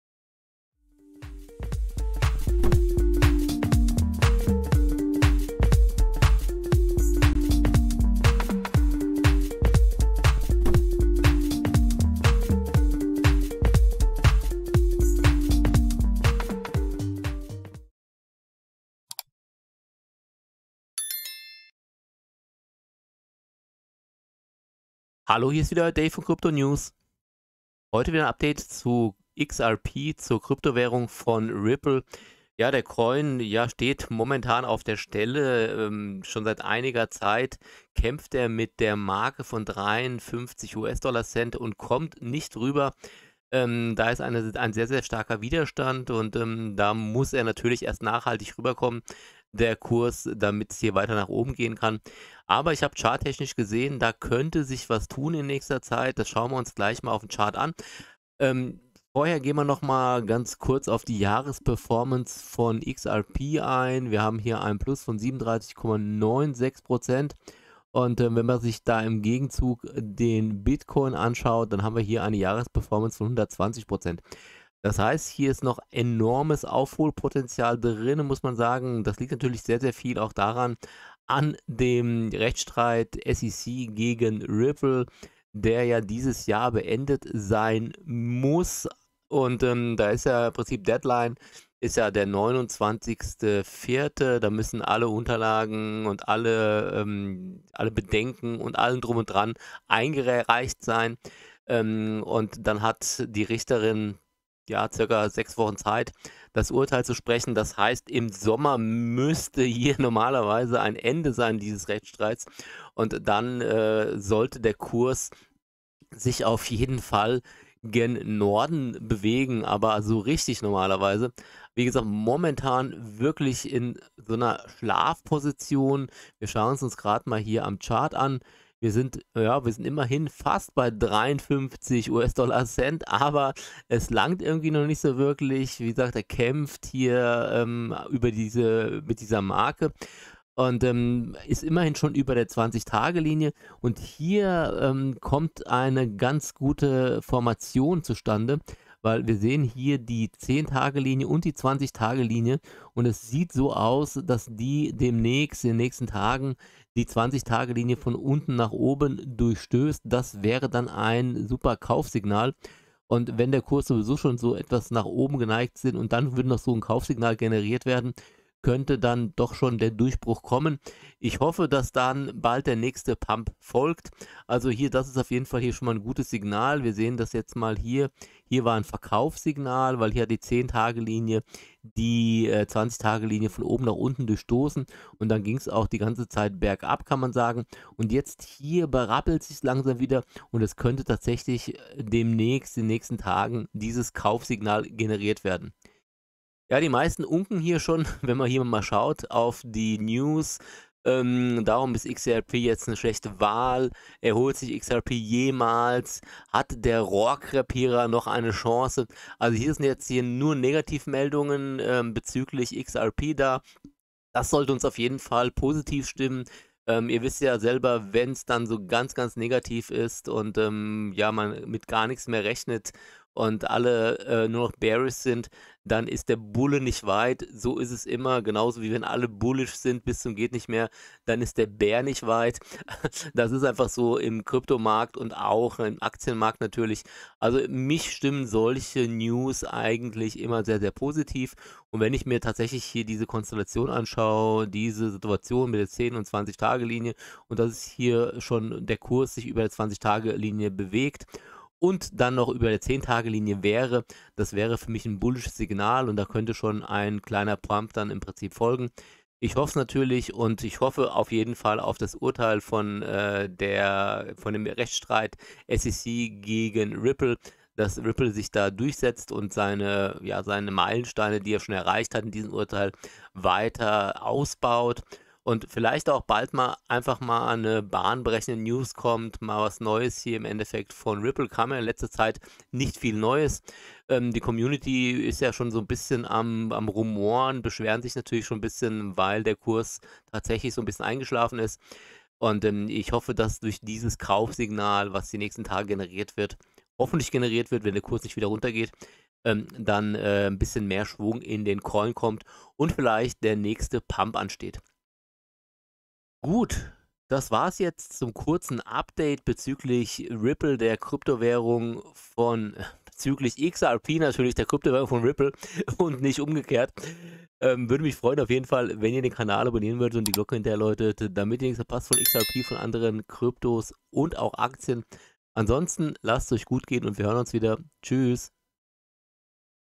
RIPPLE XRP COIN TOKEN UPDATE ✅ NÄCHSTES KAUFSINGNAL ✅ NÄCHSTER PUMP ? ✅ XRP CHARTANALYSE ✅
KRYPTO NEWS CRYPTO NEWS
Heute wieder ein Update zu XRP von Ripple 🚀🚀🚀. Was gibt es neues zum XRP COIN / TOKEN und wie verhält sich der Kurs von XRP?